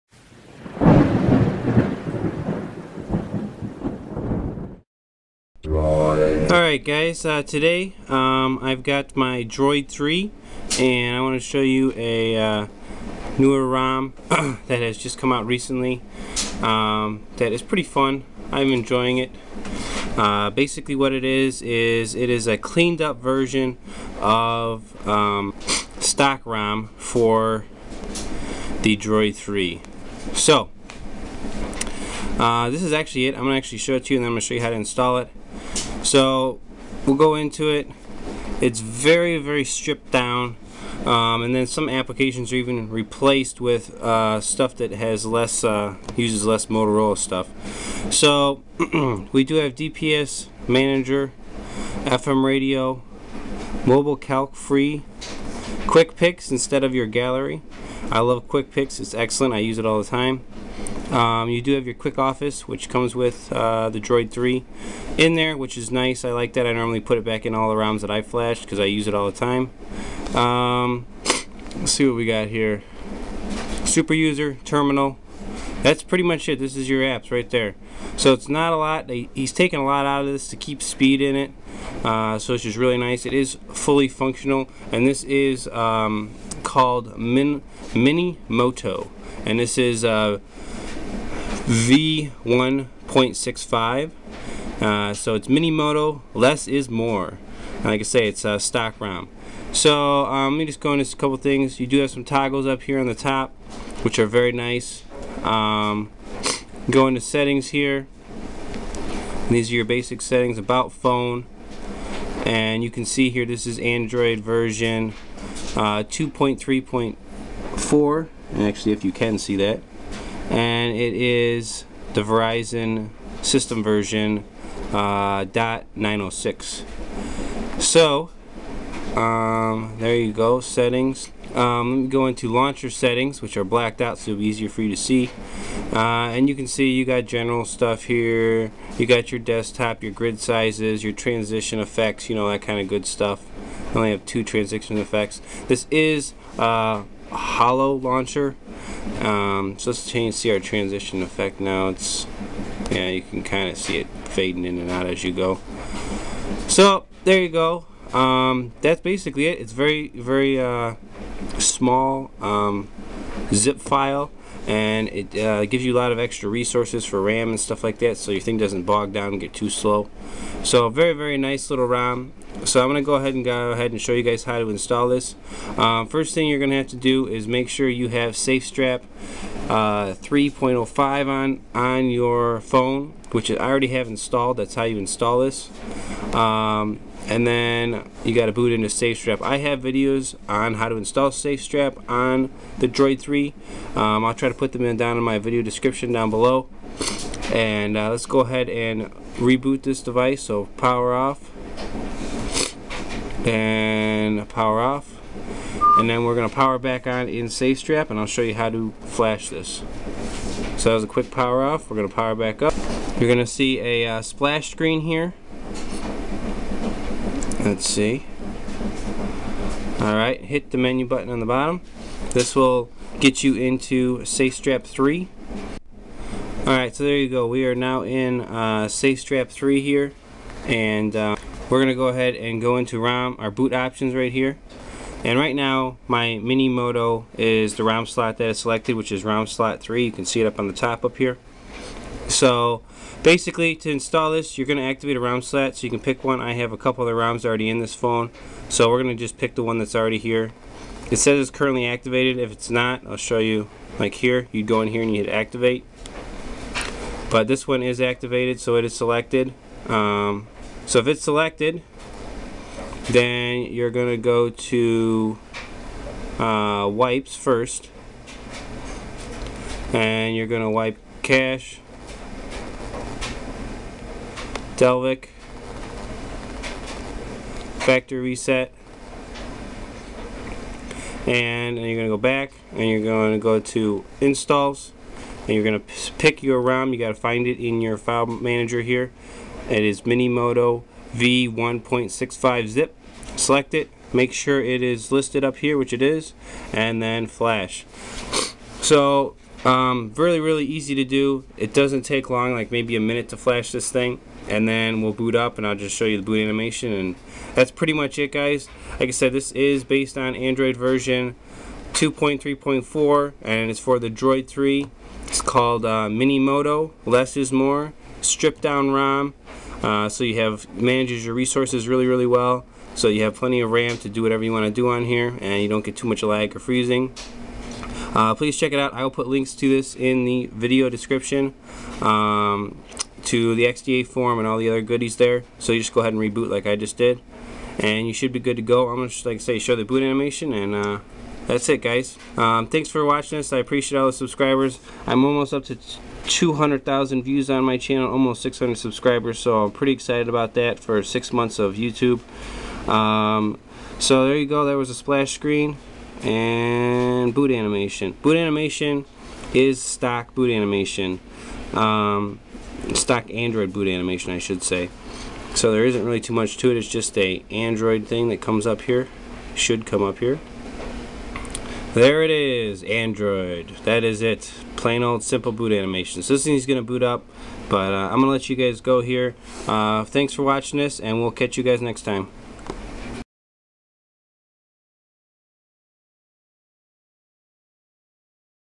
How to install Minimoto rom on the Droid 3
How to install Minimoto rom on the Droid 3 using safe strap 3.05

Requirements
droid 3
root
safe strap 3.05
rom downloaded to sd card

Link to XDA forum were rom can be found